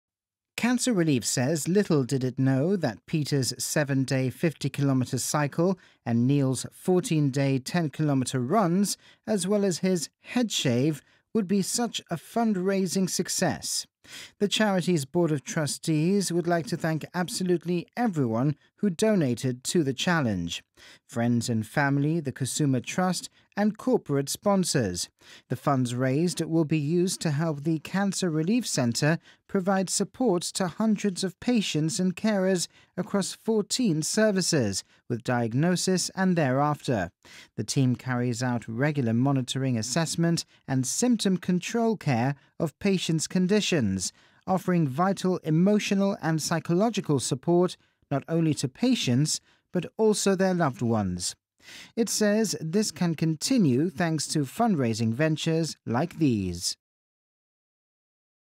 The PJ Isola Foundatio raises over a £100,000 pounds for Cancer Relief Gibraltar
The P J Isola Foundation has raised over a one hundred thousand pounds for Cancer Relief Gibraltar.  The money was raised by Peter Isola and Neil Costa in their recent gruelling Challenge, with the charity saying the amount raised by a single event is unprecedented in the 37 year history of Cancer Relief. Once again, it says, the community has come together to support the charity when the Covid 19 lockdown was having such a serious unintended knock-on effect on its regular income.